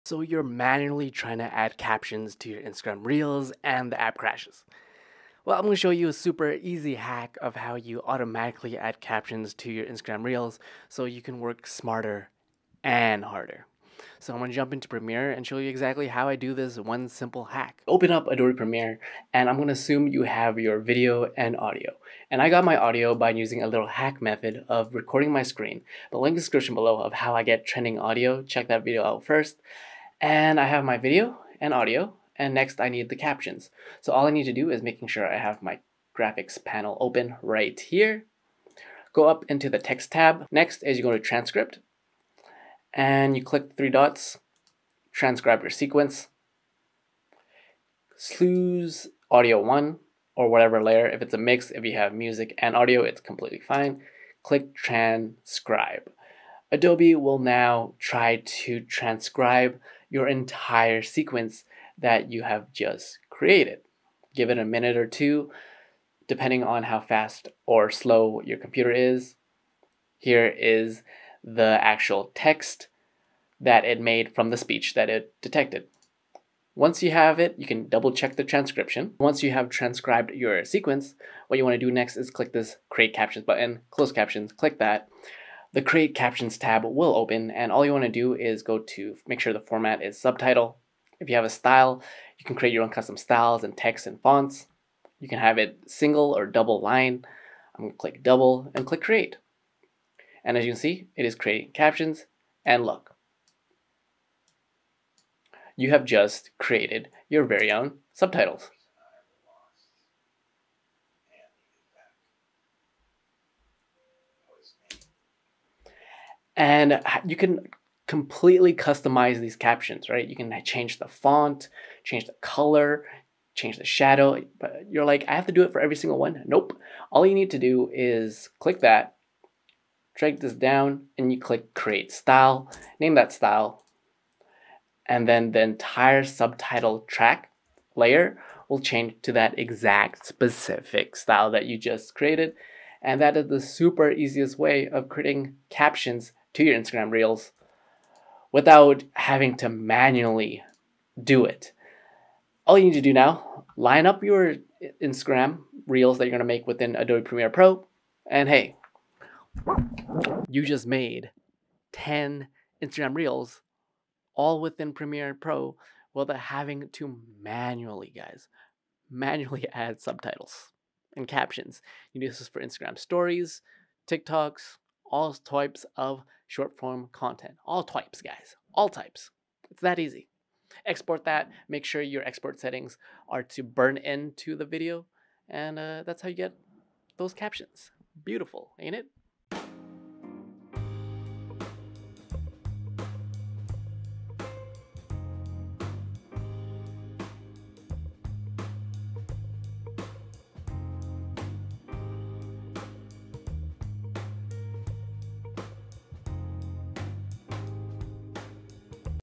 The FASTEST way to add captions to Instagram Reels (Subtitle Hack)
How do you add captions & subtitles to your Instagram Reels or stories? In this video I show you the EASIEST and fastest method in how I add these engaging captions in Premiere Pro.

📸 No idea how to use a camera?